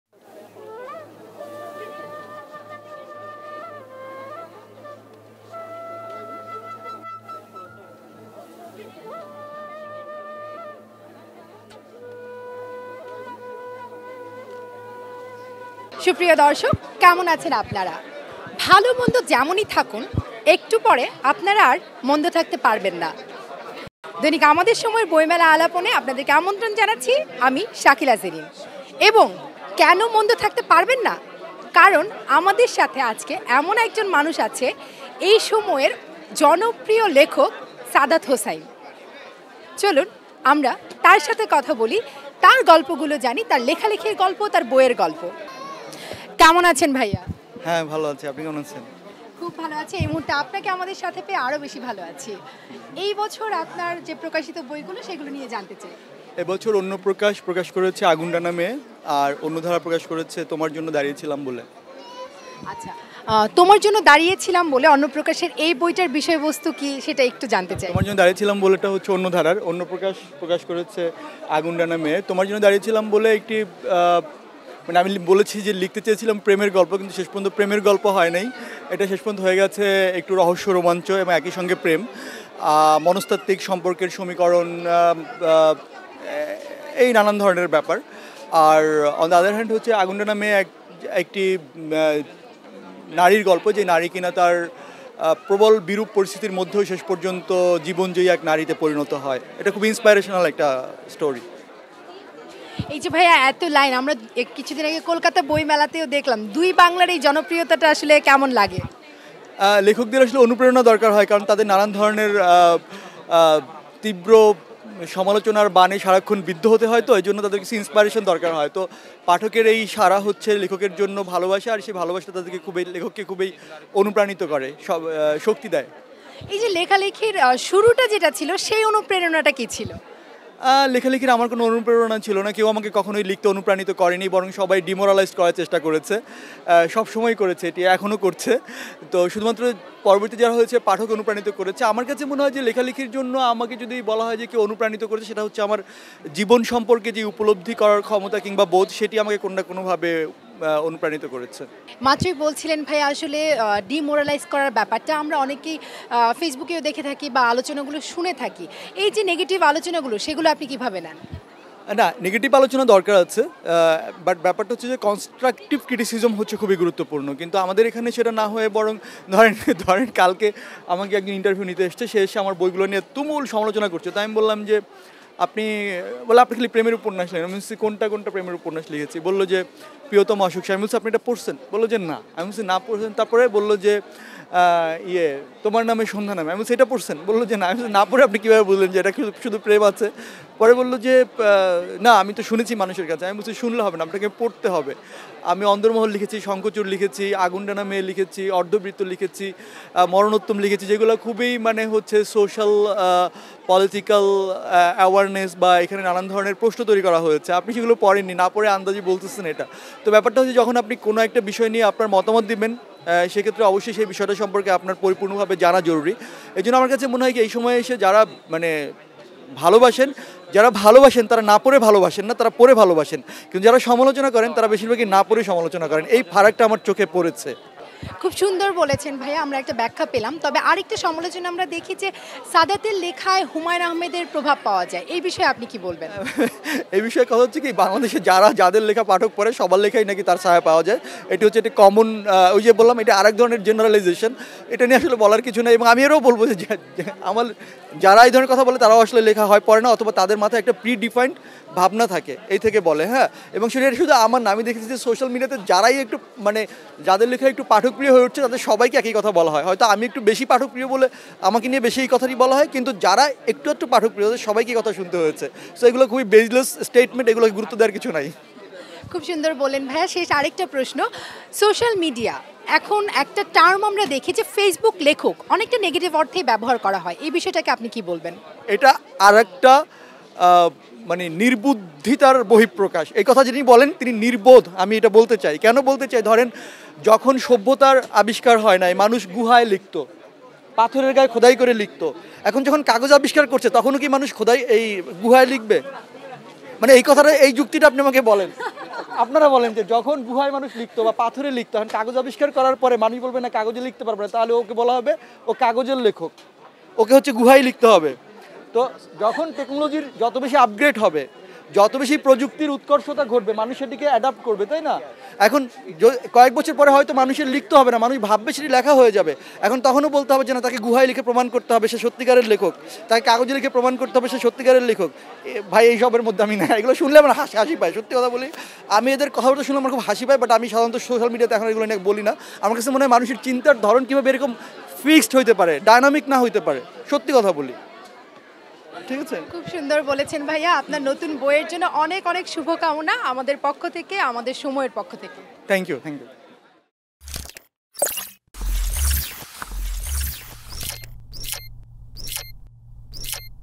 পাঠকের সাড়া হচ্ছে লেখকের জন্য ভালোবাসা: সাদাত হোসাইন | Sadat Hossain | Book Fair 2024
পাঠকের সাড়া হচ্ছে লেখকের জন্য ভালোবাসা: সাদাত হোসাইন | Sadat Hossain |  Book Fair 2024 | Amader Shomoy